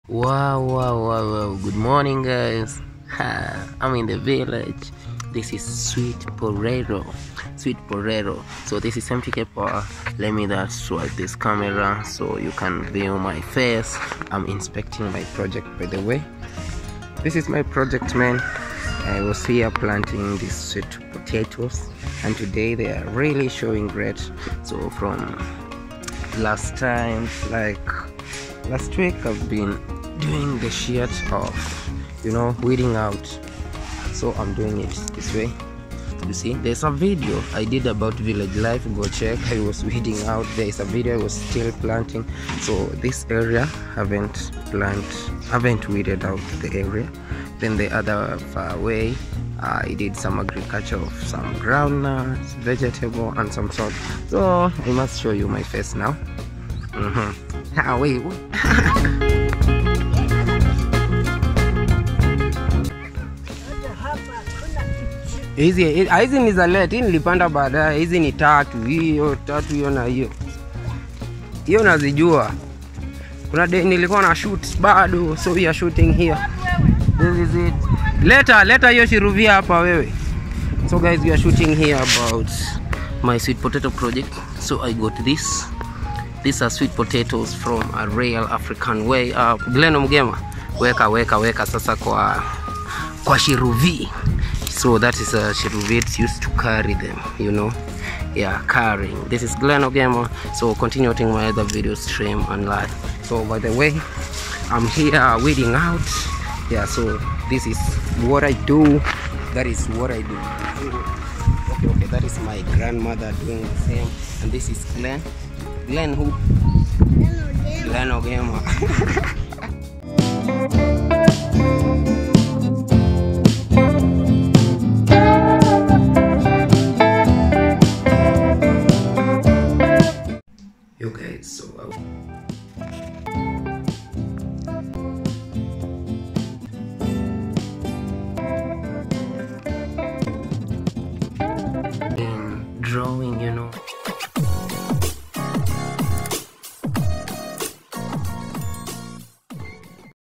Harvesting Sweet potato | African Life
hi FAM I thank you for always keep watching this amazing adventures of mtkpol... let's explore sweet potatoes in my real village home... enjoy like share and comments don't forget to watch other videos🦋